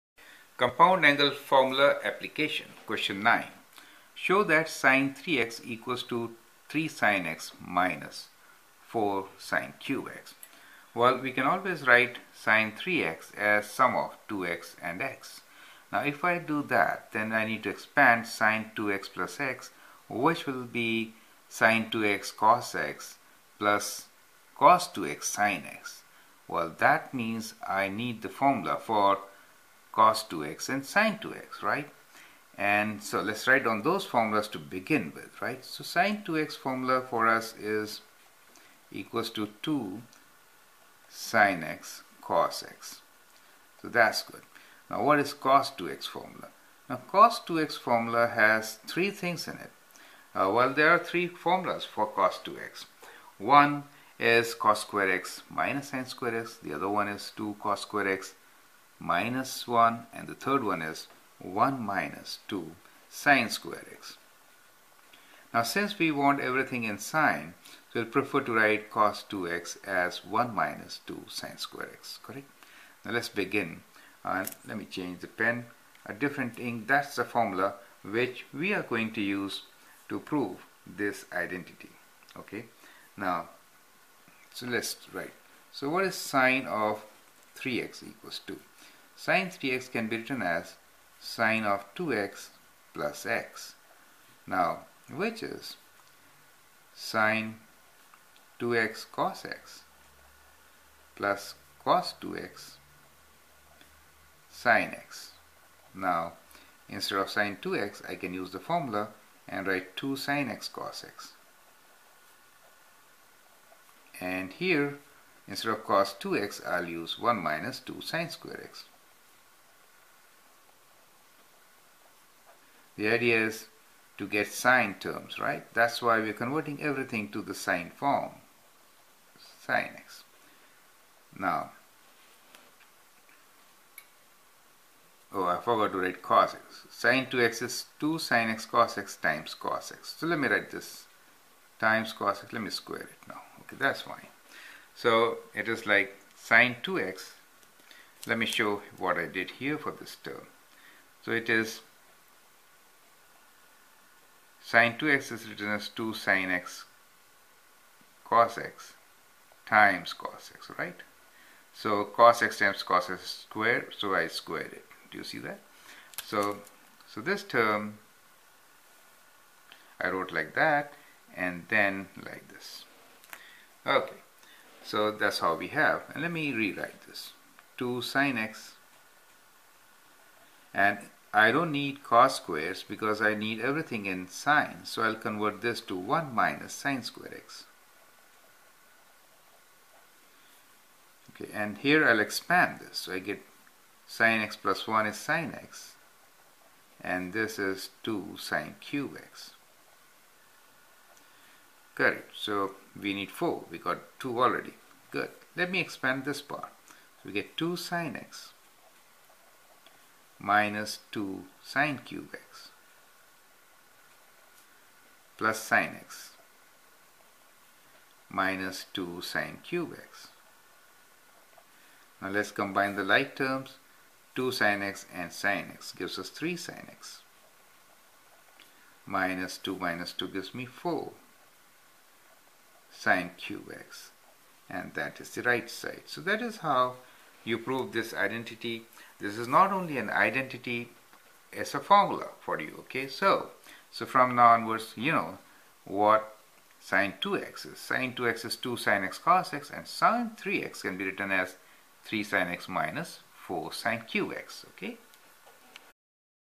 Sin 3x Compound Angle Formula Q9
This is a standard formula for sin3x and can be used to prove identities and solve trigonometric equations.New Playlist